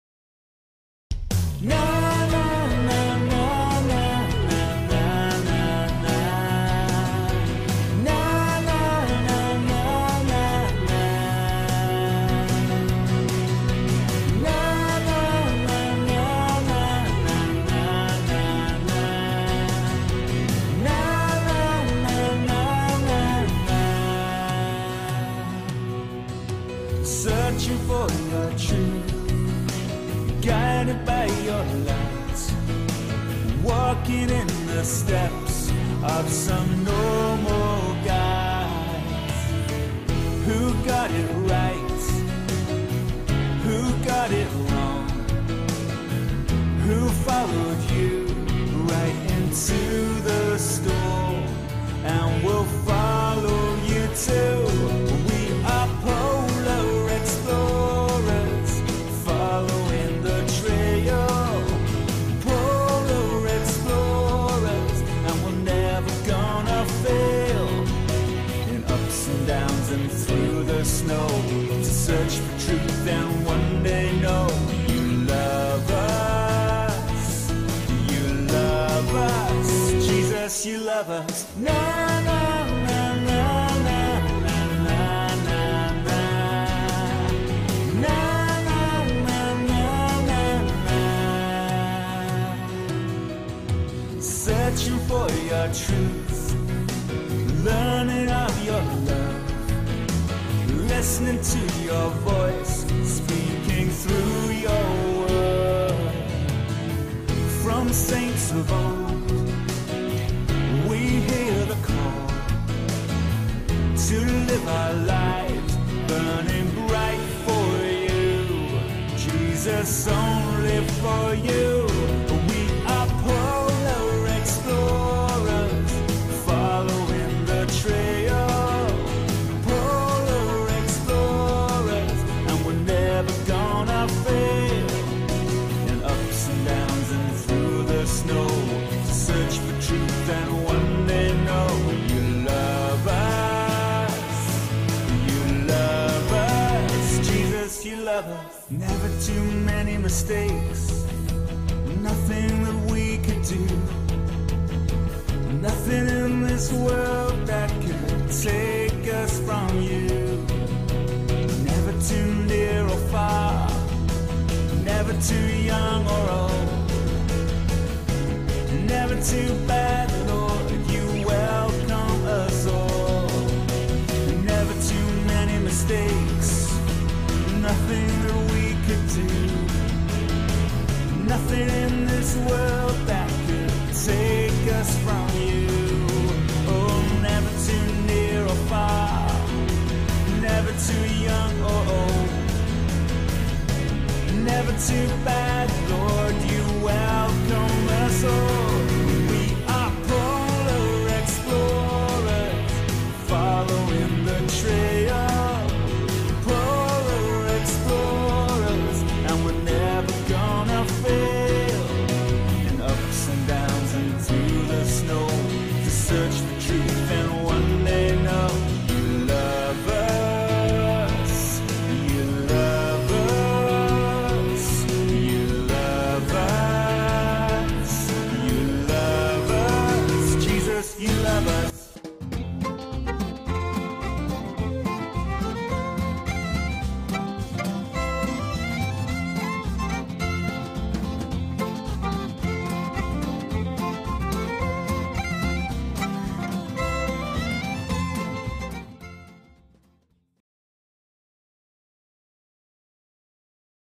2015 'Polar Explorers' Holiday Club Swanfield Chapel. Chichester. UK
Holiday Clubs are 2 hours of fun! Games, competitions,  Bible stories,  Crafts & activities all based around a fun theme.